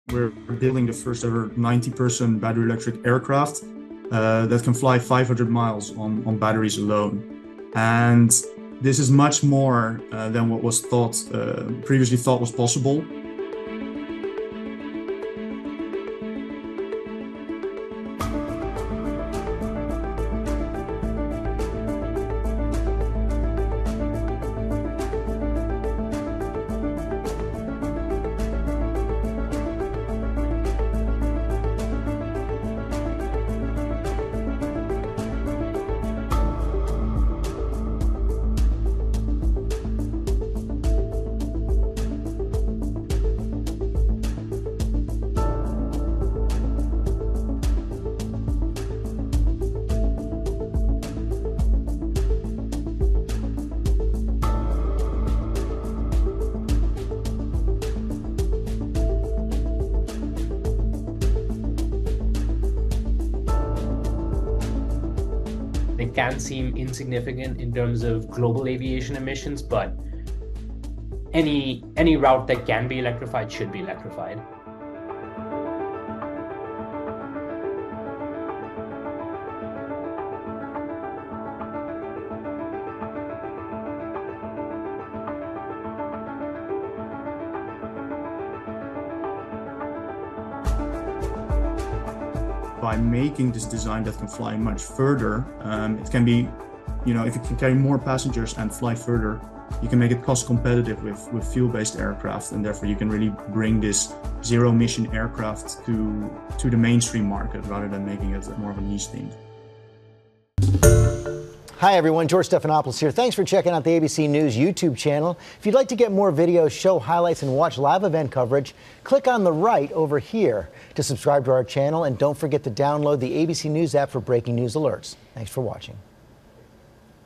Electrified air travel could be here sooner than we think
Aviation startup Elysian is currently developing a battery-powered aircraft that could carry 90 passengers farther than 500 miles.